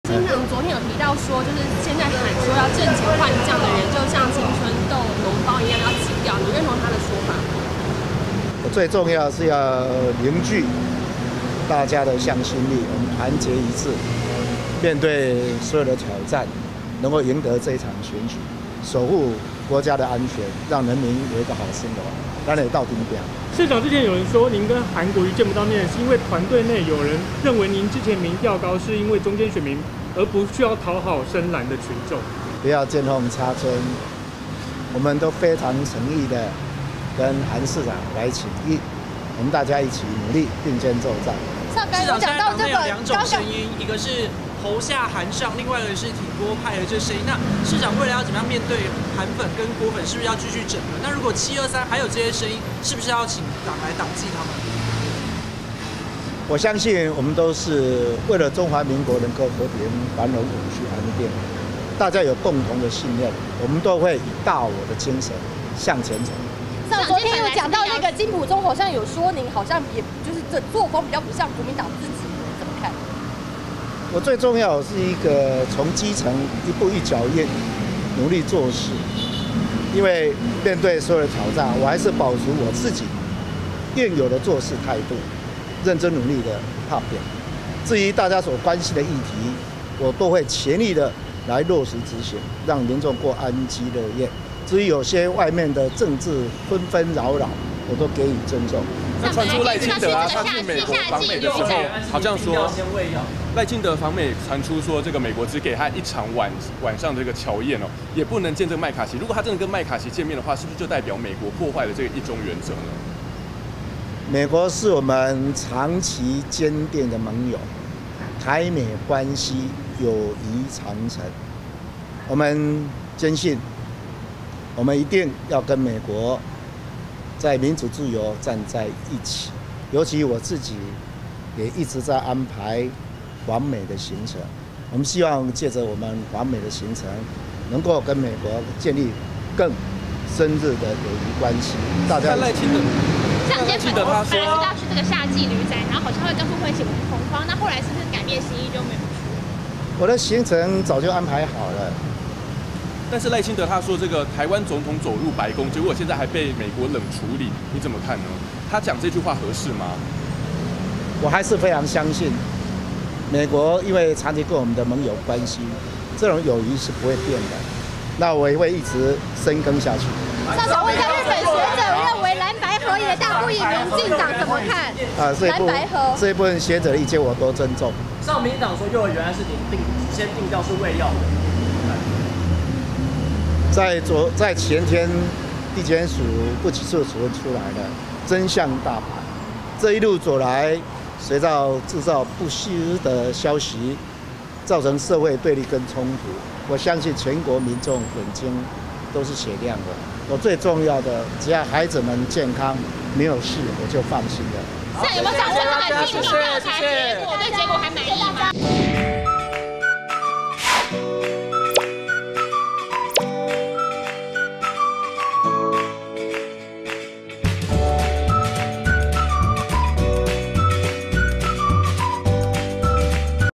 侯友宜回應金溥聰"膿包說"! "四個字"淡定看待"換侯"雜音
責任小編:成編/佳仁編